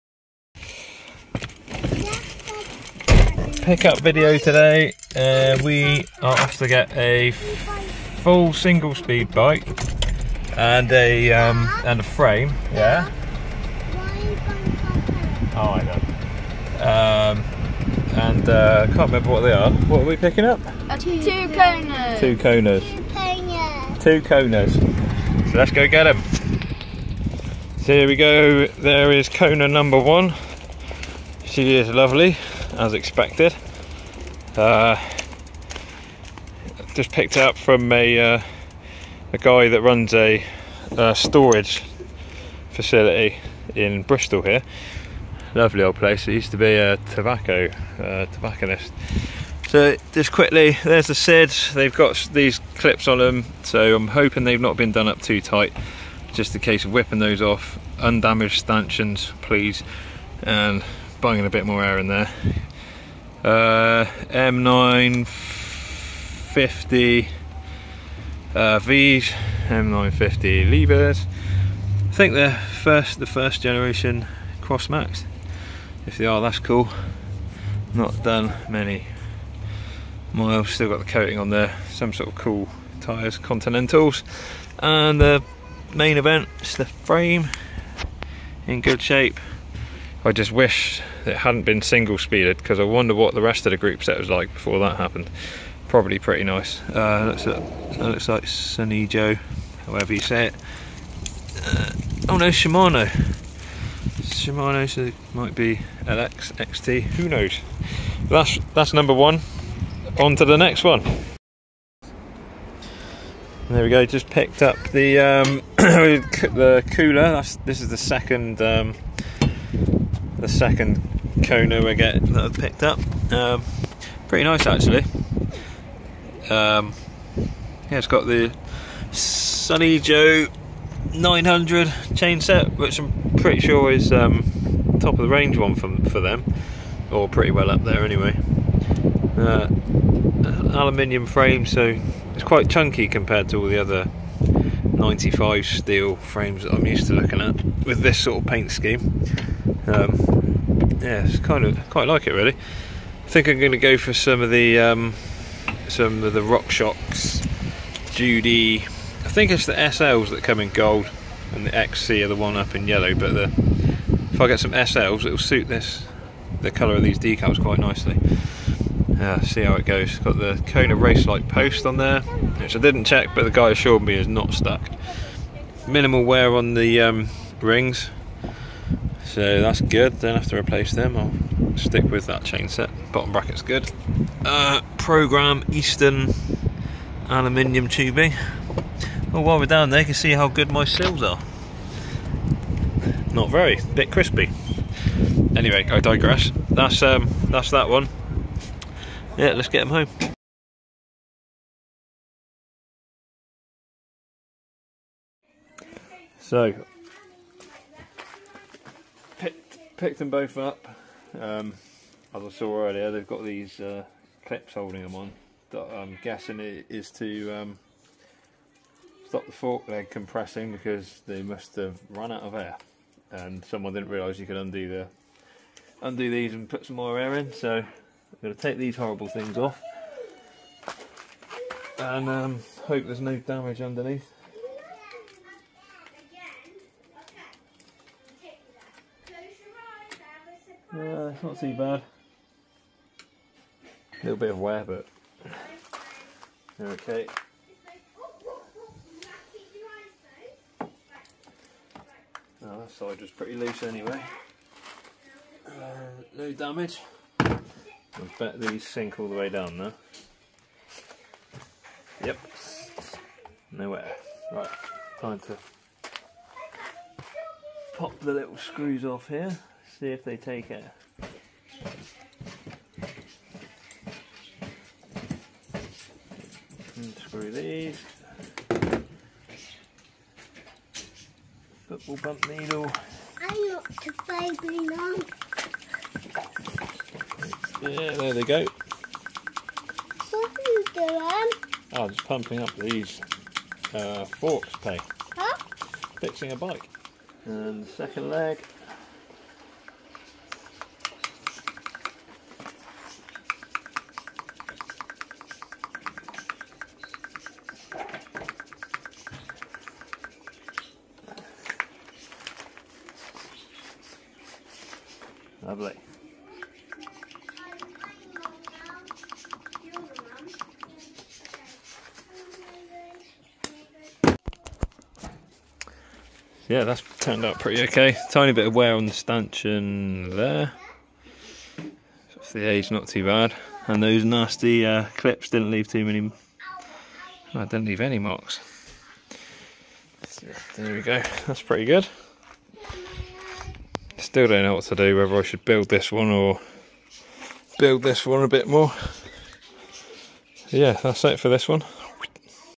Part 1 Kona Kula, M950 XTR, TWO retro Kona mountain bikes. Start of a new project 😀
This is the first part in the series on my Kona Kula. As you will see I bought it as just a frame, now to start the fun part, the build.